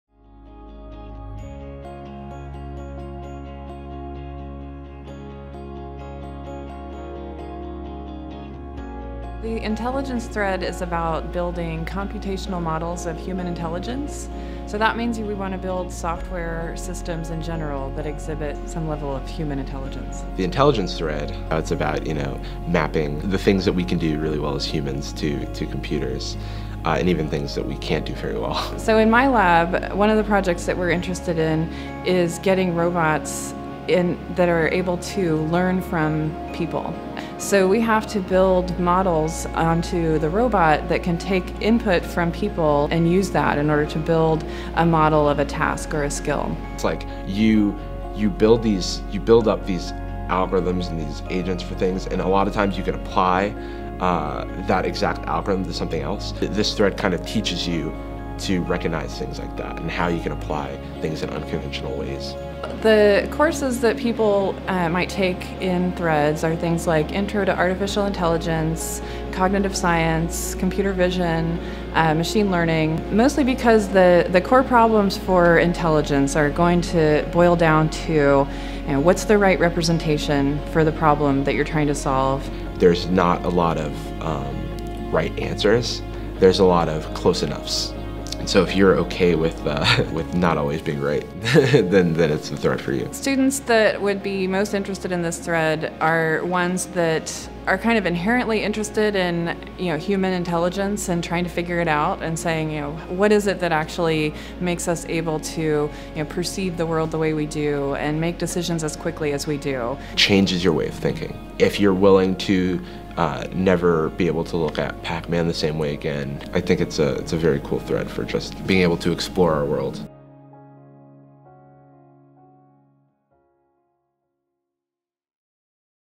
Intelligence
The Intelligence thread focuses on the development of top-to-bottom models of human-level intelligence.

Videos Filmed and Produced By: Tandem Bike Media